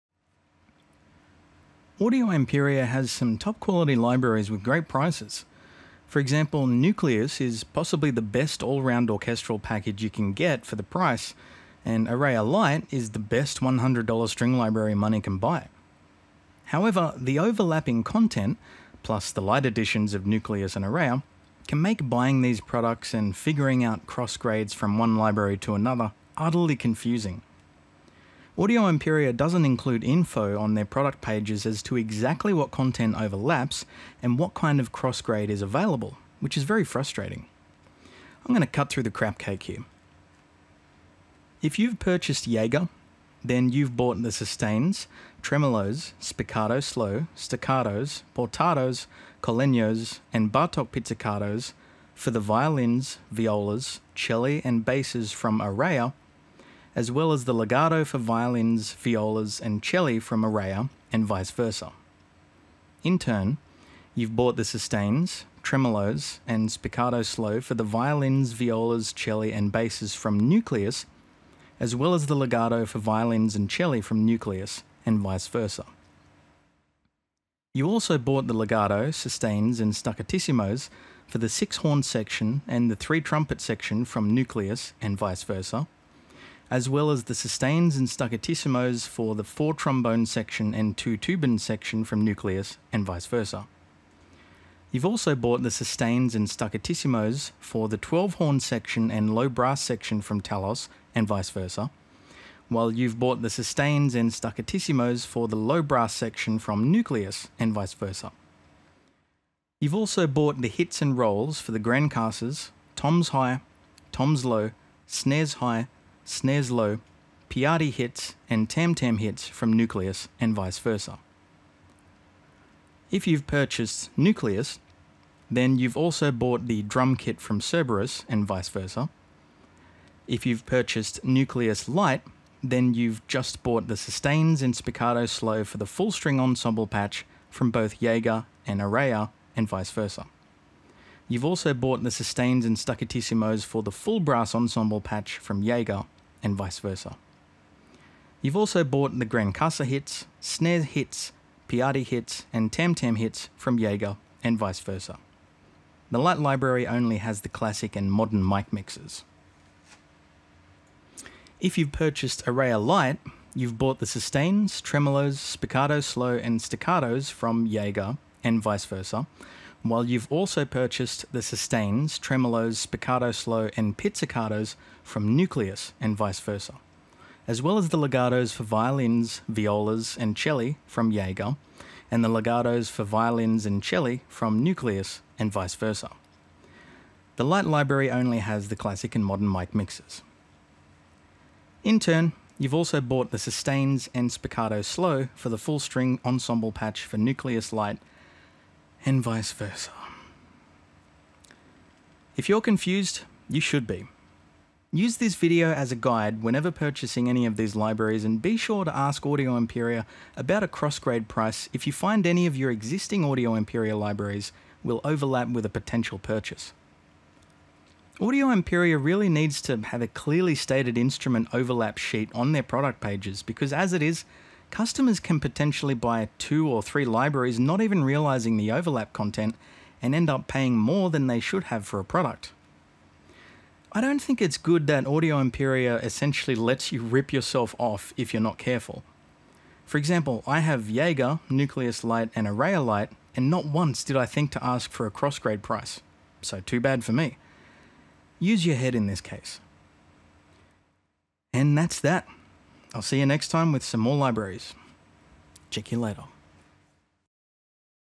Explaining Audio Imperia's Libraries & Overlapping Content
A PSA for customers who suffer from TLDR.